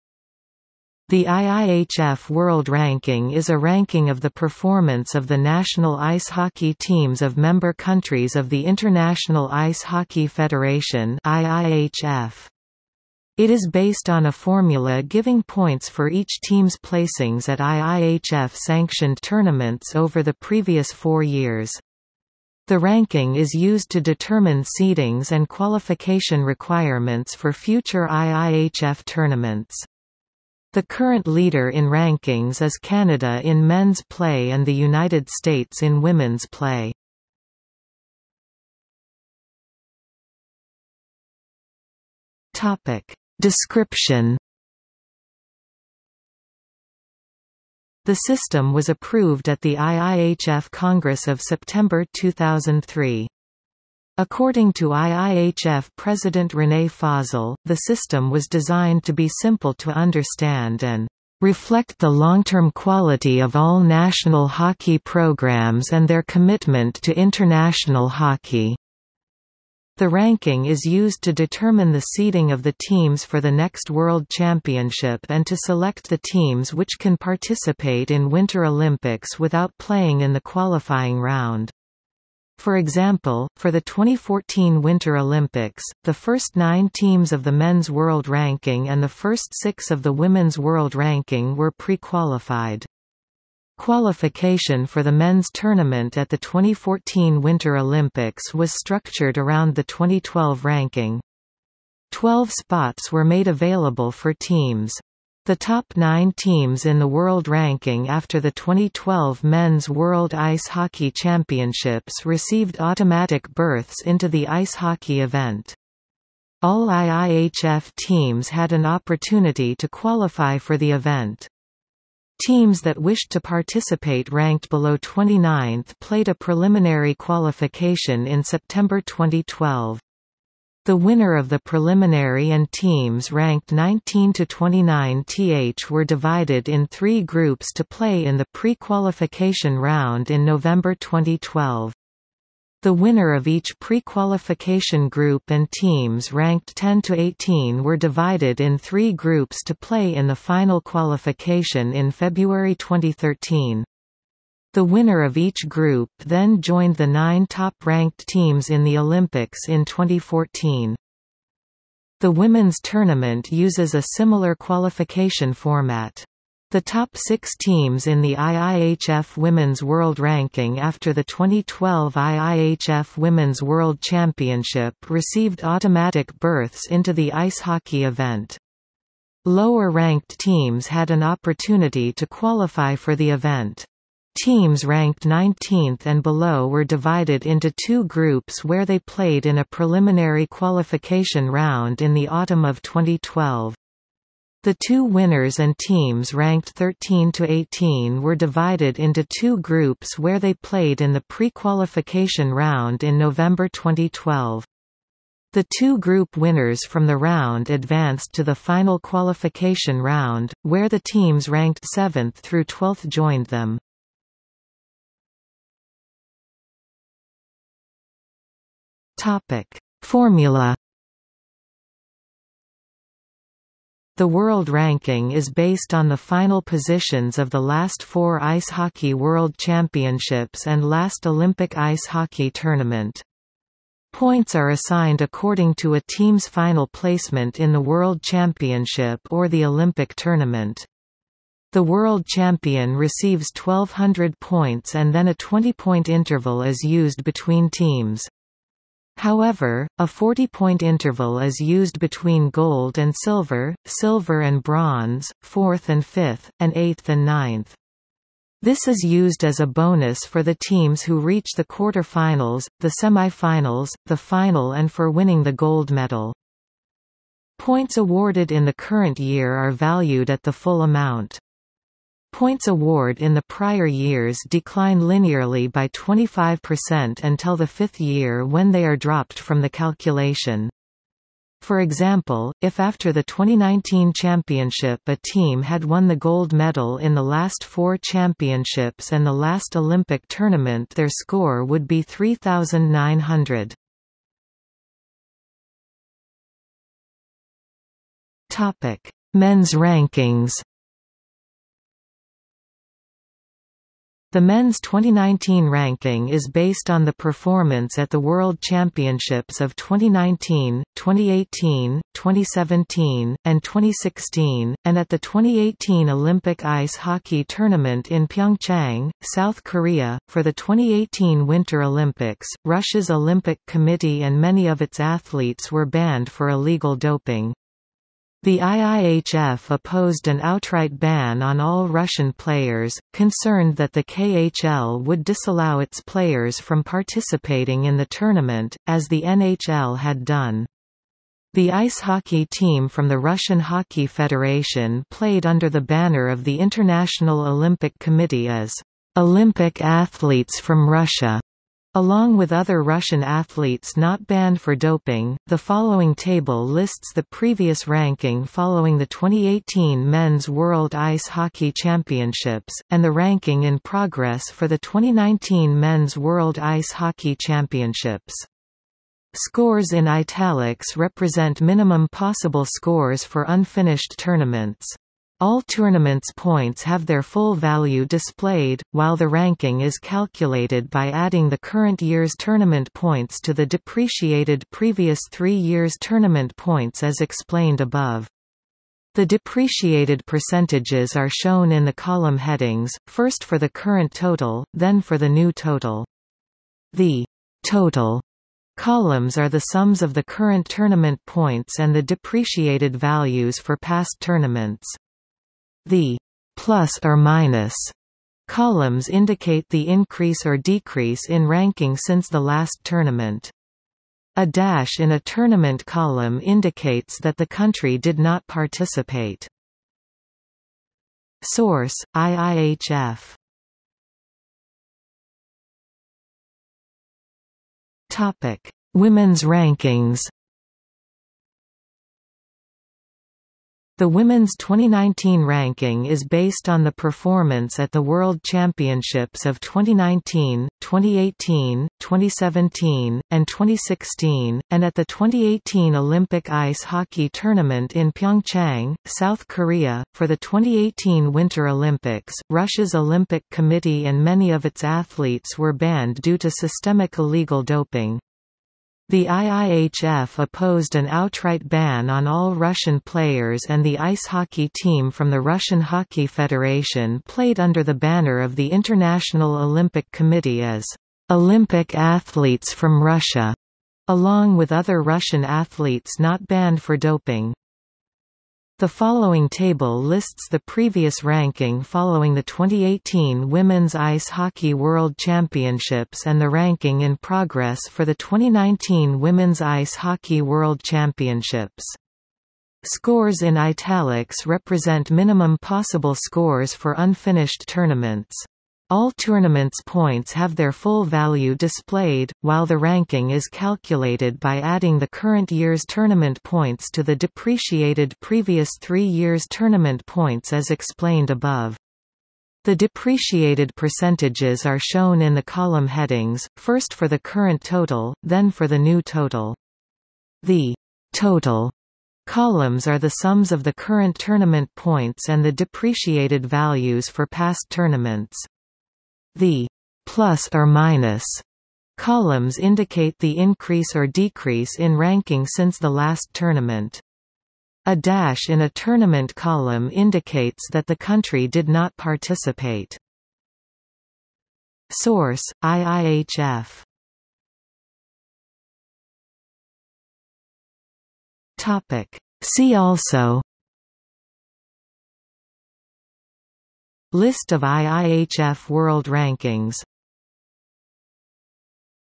IIHF World Ranking | Wikipedia audio article
00:00:38 1 Description
00:03:35 1.1 Formula
00:04:54 2 Men's rankings
00:07:04 3 Women's rankings
00:09:08 4 See also

- Socrates

SUMMARY
The IIHF World Ranking is a ranking of the performance of the national ice hockey teams of member countries of the International Ice Hockey Federation (IIHF). It is based on a formula giving points for each team's placings at IIHF-sanctioned tournaments over the previous four years. The ranking is used to determine seedings and qualification requirements for future IIHF tournaments. The current leader in rankings is Canada in men's play and the United States in women's play.